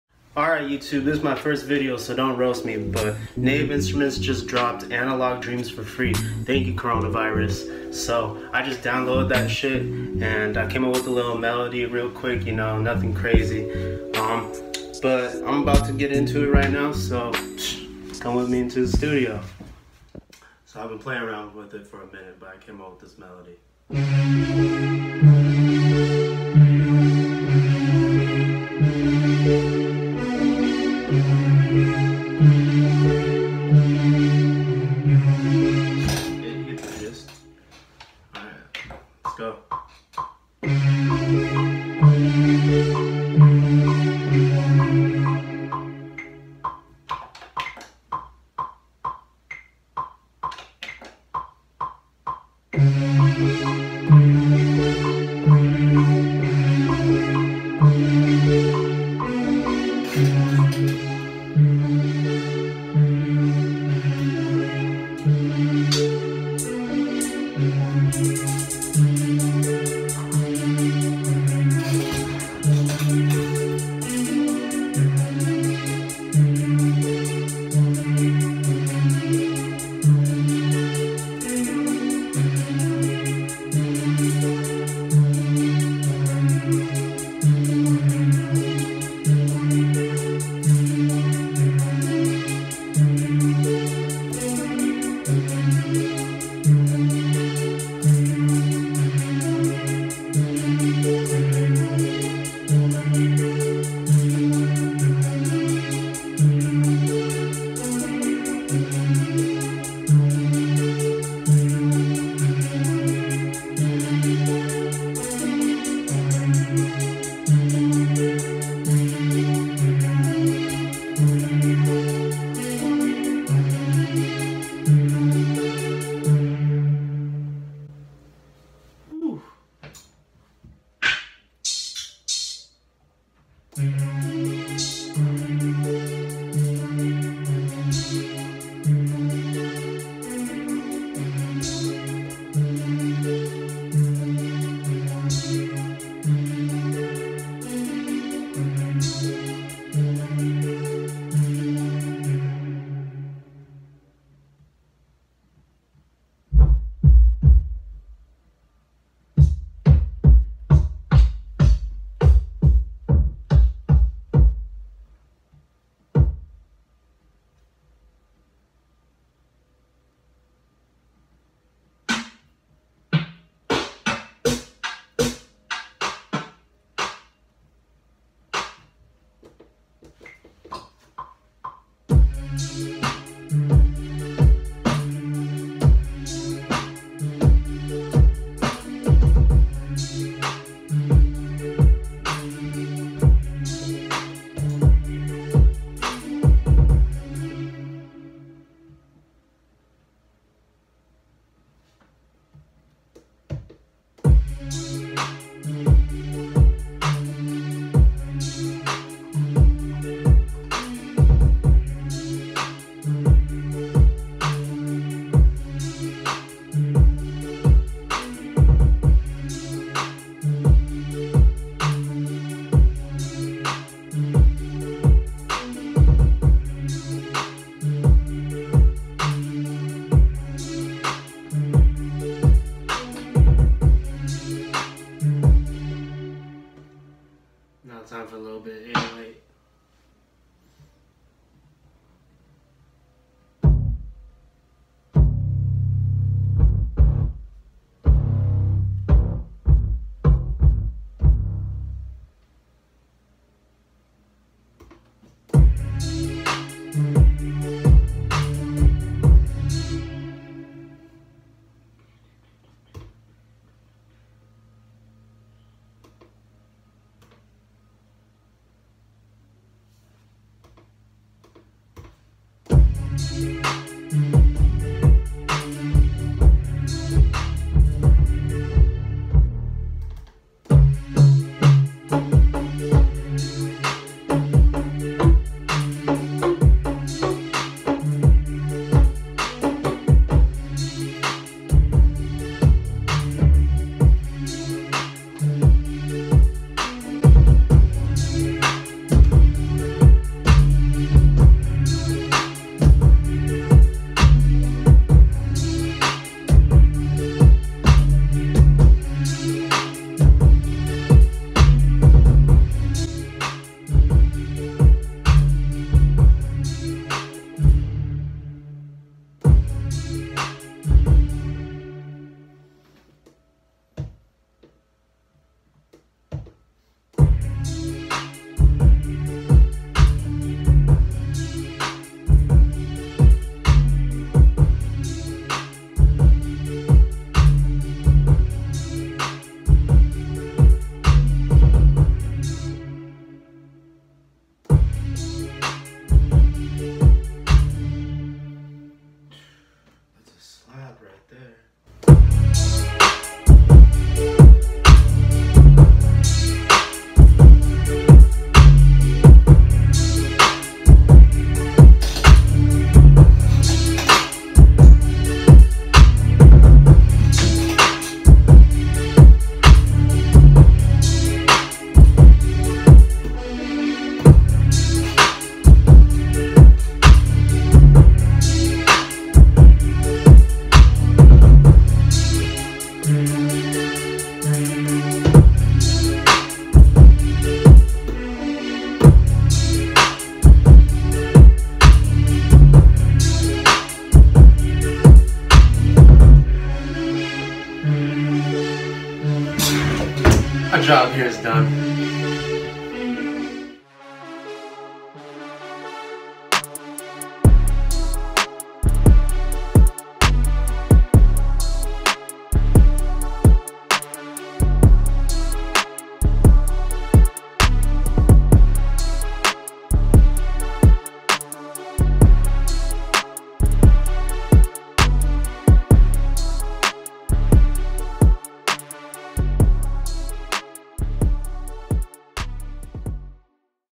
FREE Analog Dreams?! (thanks corona virus) Making a Fire Beat in 5 Minutes
Analog Dreams is free until the end of the month so heres a link for the producers